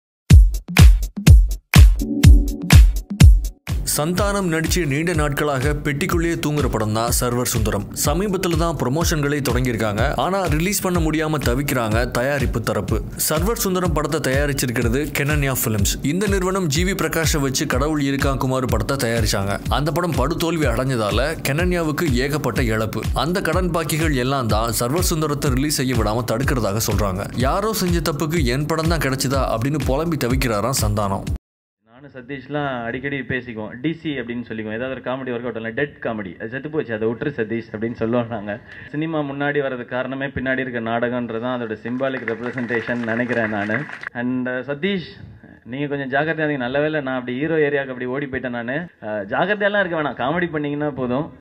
"Why My movie" - Santhanam's angry statement on G.V. Prakash movie loss | Server Sundaram Problem
"Why My movie" - Santhanam's angry statement on G.V. Prakash movie loss | Cine Flick